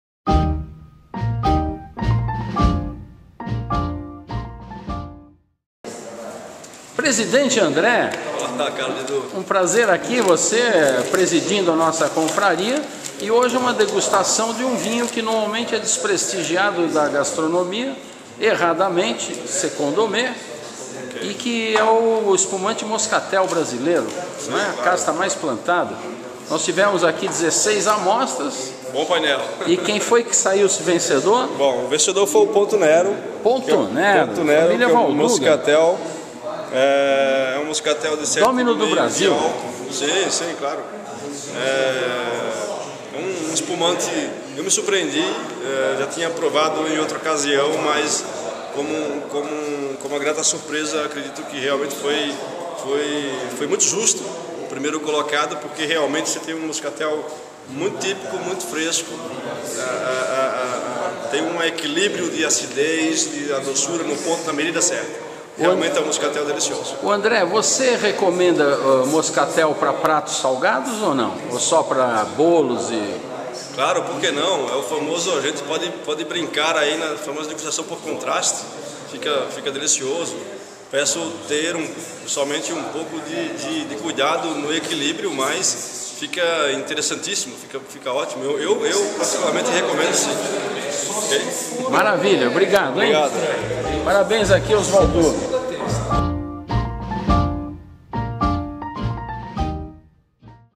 Confraria dos Sommeliers - Ponto Nero Moscatel.mov
Ponto Nero Moscatel da Domno do Brasil vence degustaçnao às cegas da Confraria dos Sommeliers entre 16 amostras de alta qualidade. 2011